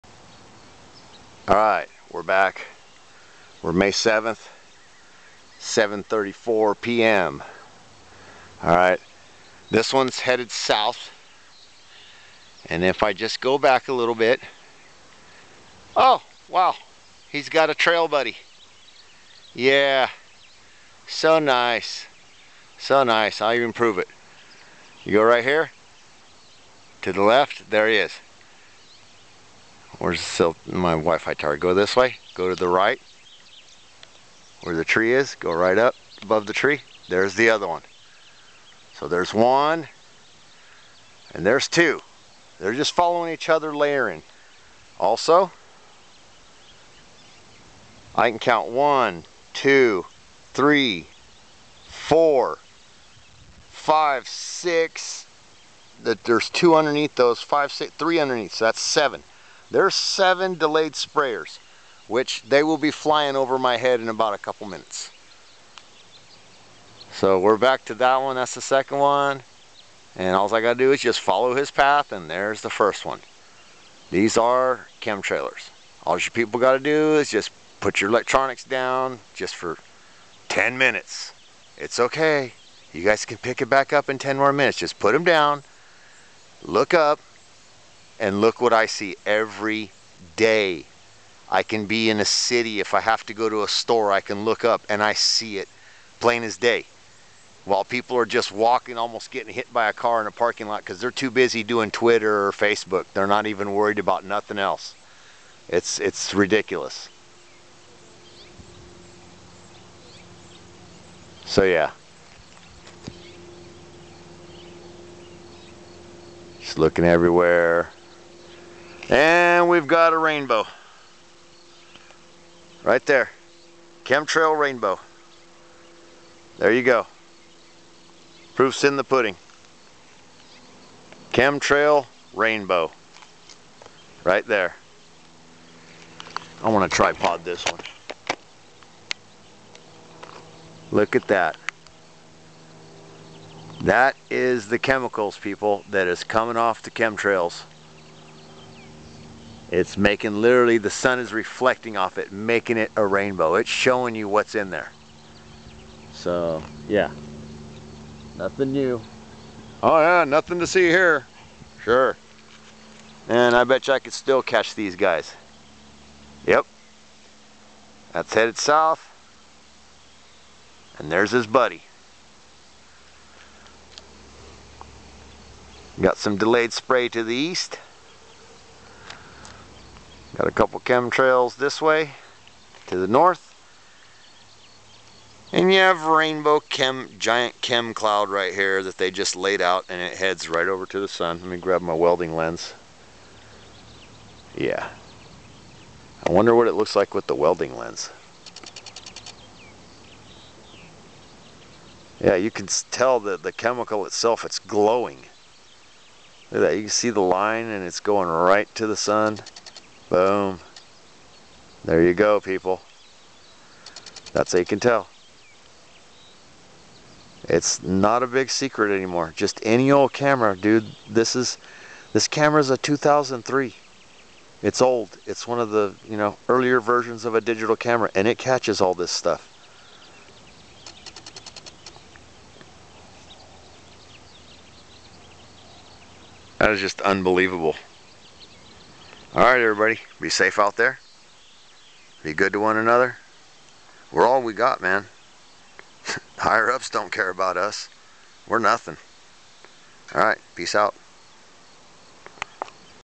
2-Chemtrailers following each other/7-delayed sprayer puffs/Chemtrail Rainbow 5/7/18
Taken on 5/7/18 @ 7:34 PM. Observing 2-chemtrailers following each other.